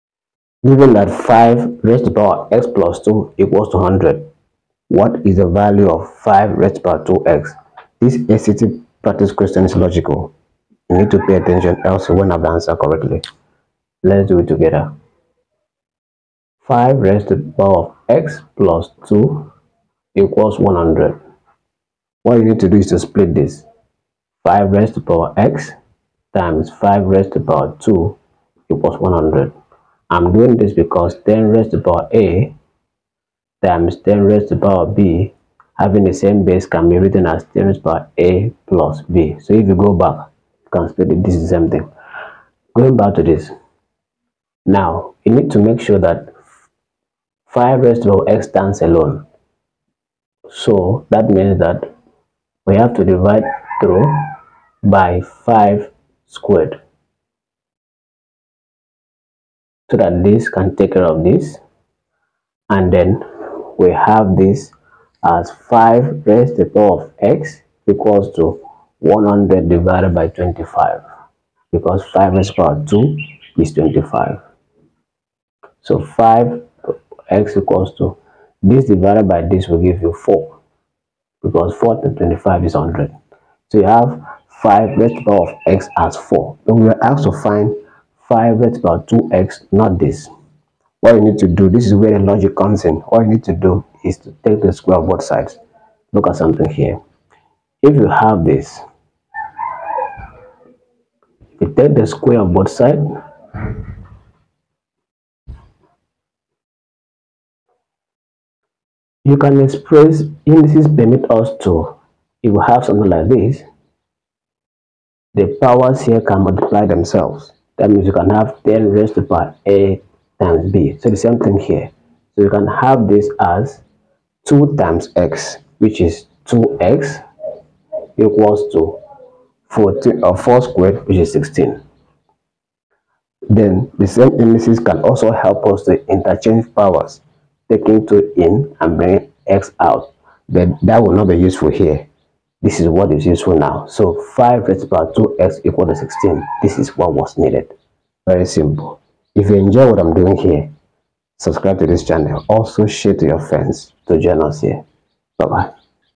SAT Math Practice Solve Exponential Equation Quickly
Master exponential equations with this quick SAT math practice problem. Boost your problem-solving speed and get exam-ready with clear step-by-step solutions.